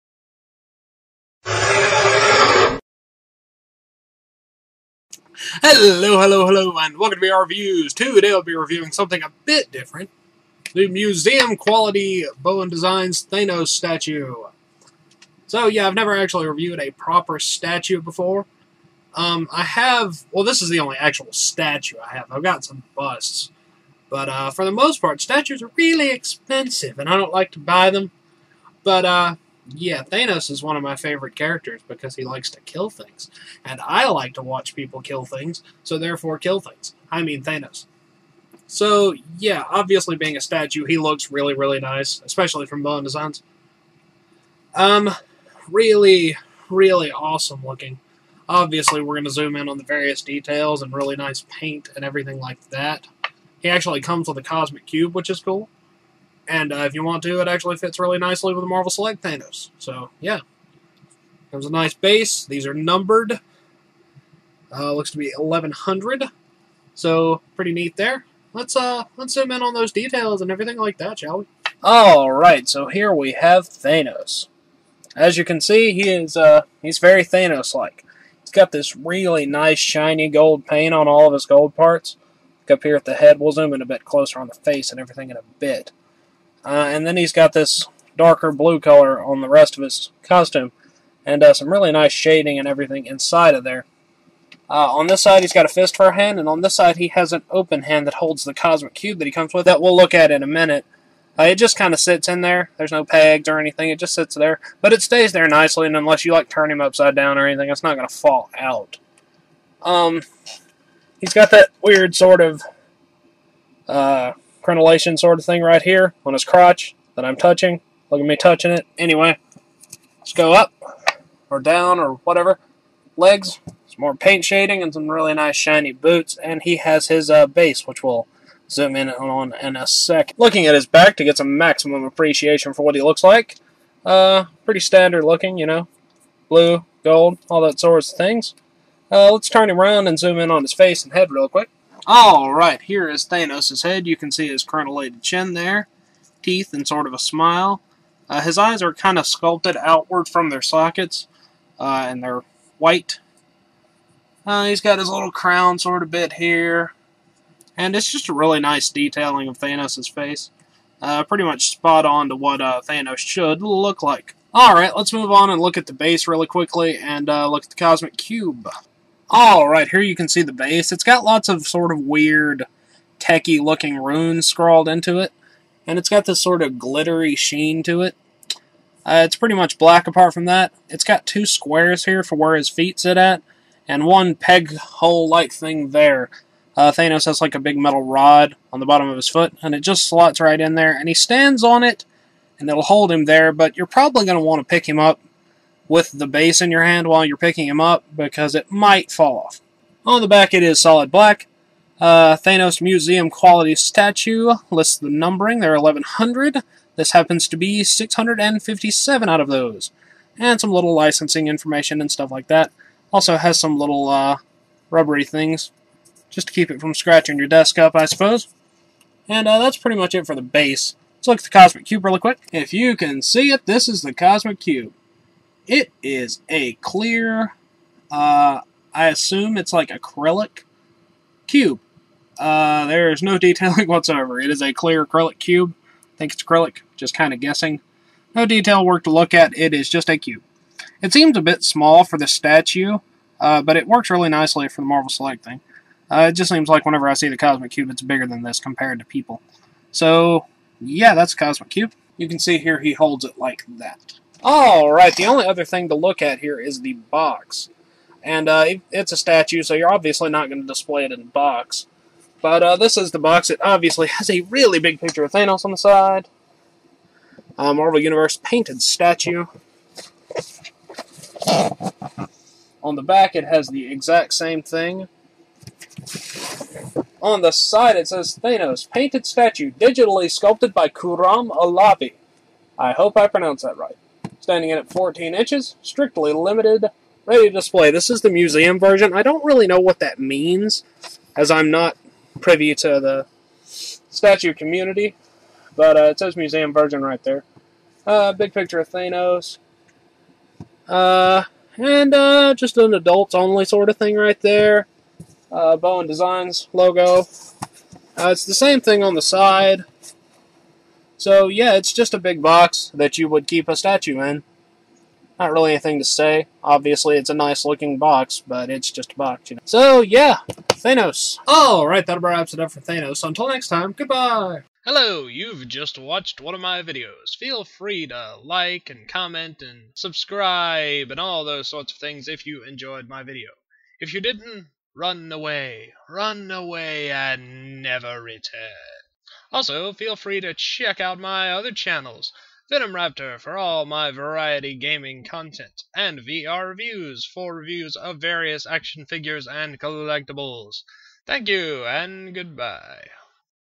VR Reviews: Bowen Designs- "Museum Quality" Thanos Statue Review
I'd like to see a museum that had a statue of Thanos.